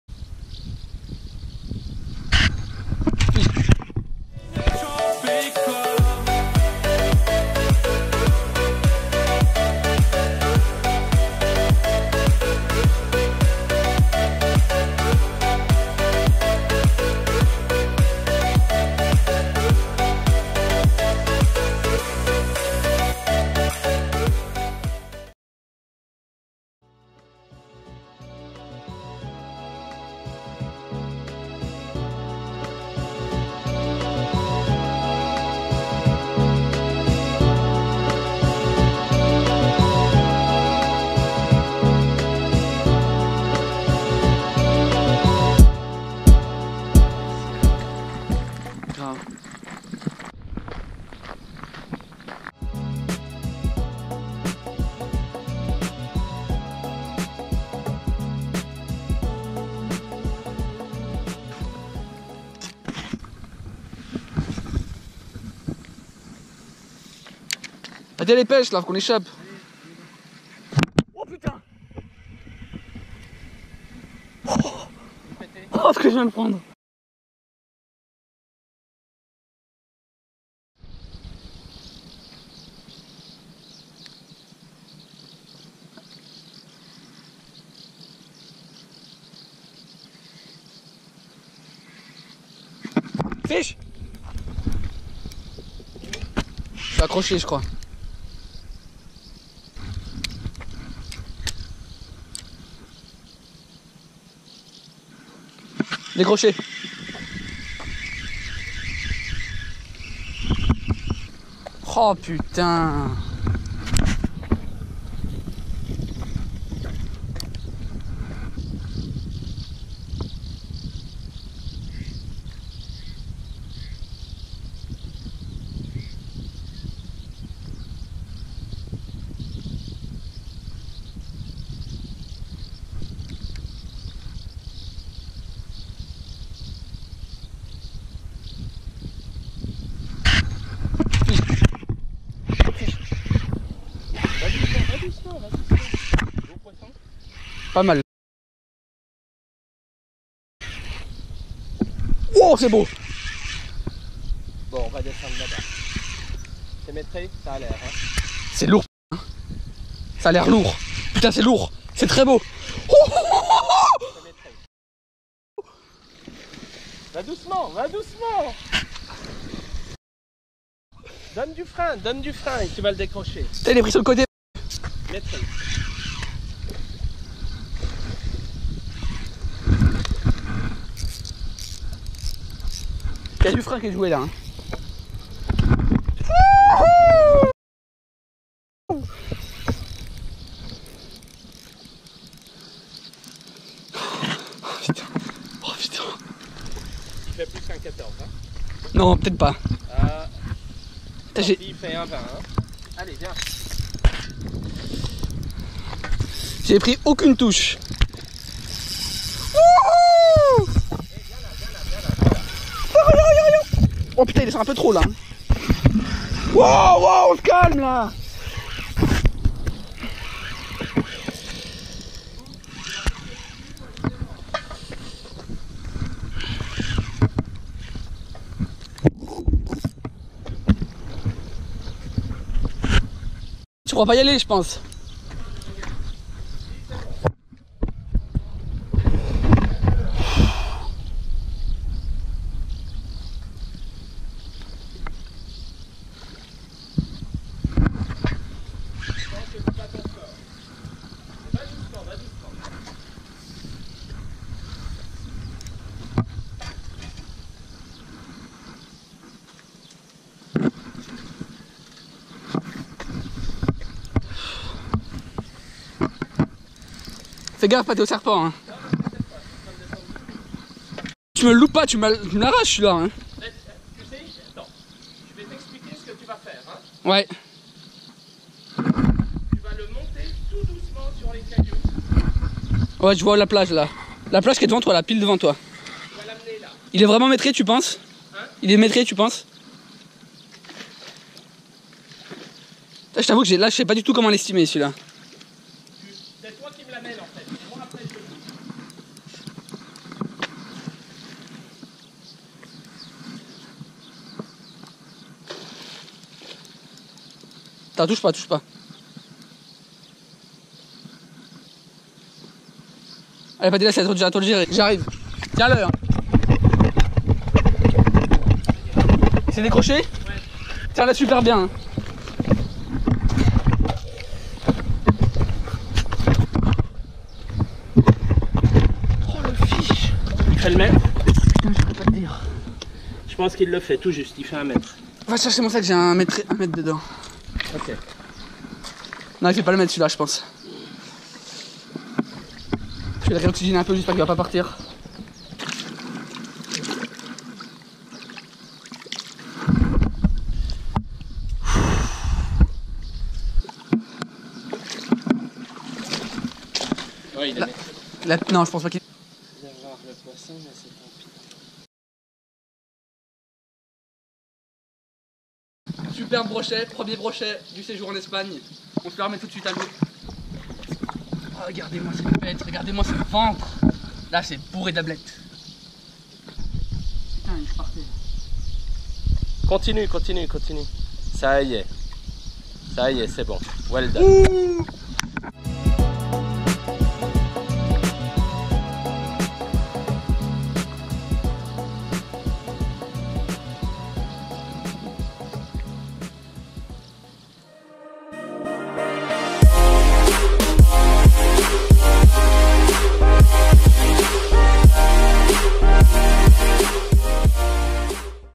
Gros Brochets en Espagne - Jour 0
Premier jour de l'arrivée en Espagne pour un séjour de pêche aux brochets avec l'équipe de Pesca Extremadura, juste arriver on part de suite pour un coup du soir !
Gros poissons au rendez-vous ?